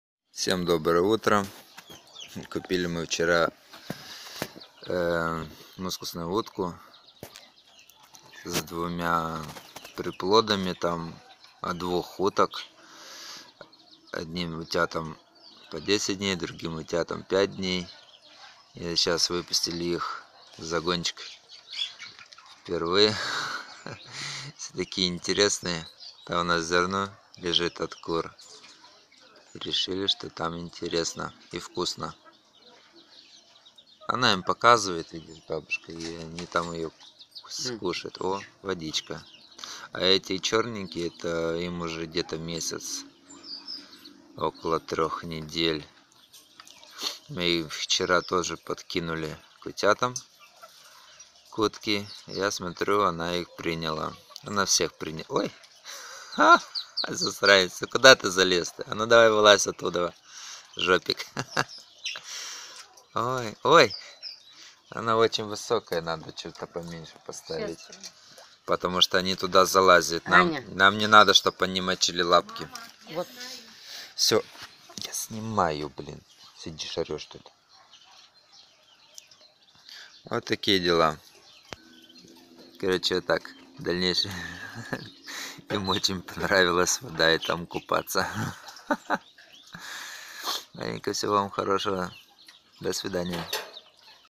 Мускусная утка с приплодом.
12 сентября, приобрели новых жильцов.